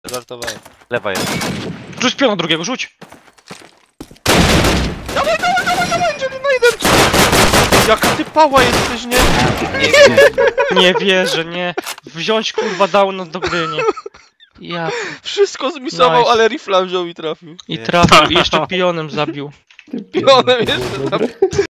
why his team mates abuse him instead of congratulate him?!
a beautiful clutch 1v2, all planned, added subs for your convenience